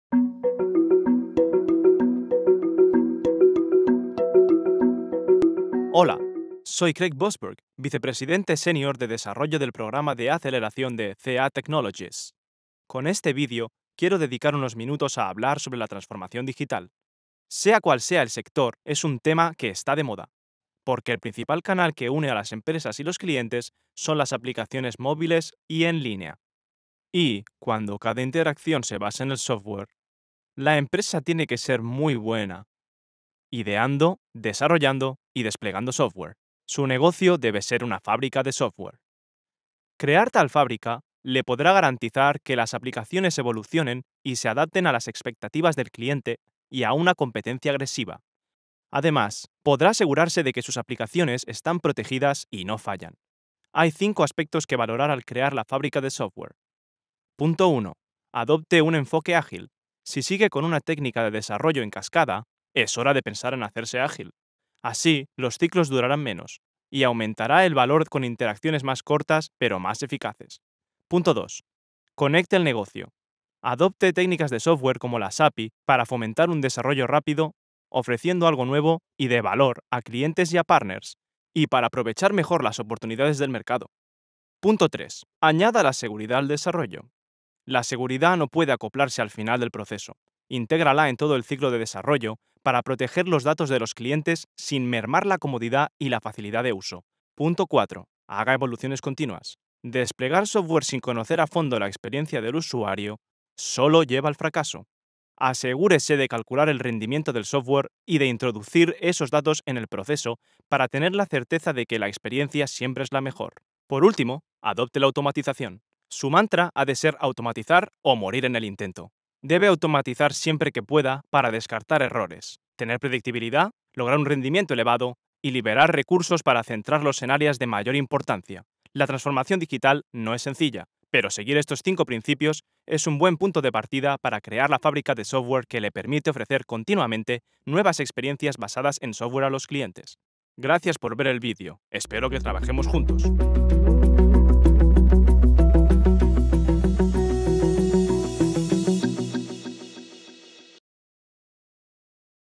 Las bases de la transformación digital: razones para pensar como una empresa de software
Los negocios actuales necesitan una fábrica de software moderna para desarrollar y distribuir software más rápido y así satisfacer las necesidades del cliente. Tener una fábrica de software ya no es opcional: es necesaria para poder alcanzar el éxito en la transformación digital. Craig Vosburgh propone cinco elementos que se deben tener en cuenta a la hora de crear la fábrica de software de una organización.

Aprenda a construir su propia fábrica de software:
ca.com/digitally-remastered